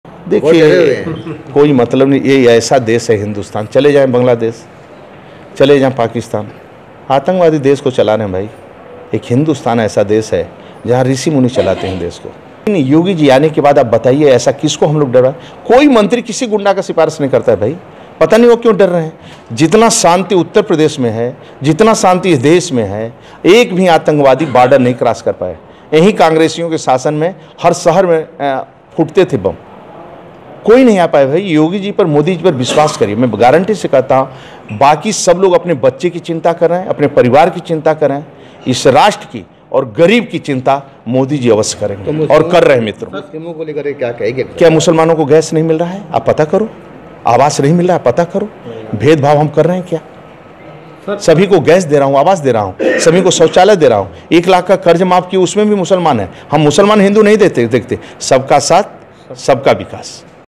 'हमारा कोई भी मंत्री 'गुंडे' की सिफारिश नहीं करता, फिर क्यों डर रहे इकबाल अंसारी'
झांसी : एक ओर जहां मुस्लिम पक्षकार इकबाल अंसारी के अयोध्या छोड़ने की धमकी के बाद प्रशासन ने उनकी सुरक्षा बढ़ा दी है. वहीं दूसरी ओर झांसी पहुंचे परिवहन मंत्री स्वतंत्र देव सिंह ने कहा कि हिंदुस्तान को संत चलाते हैं. हमारा कोई भी मंत्री किसी भी गुंडे की सिफारिश नहीं करता है. फिर भी इकबाल अंसारी इतना क्यों डर रहे हैं.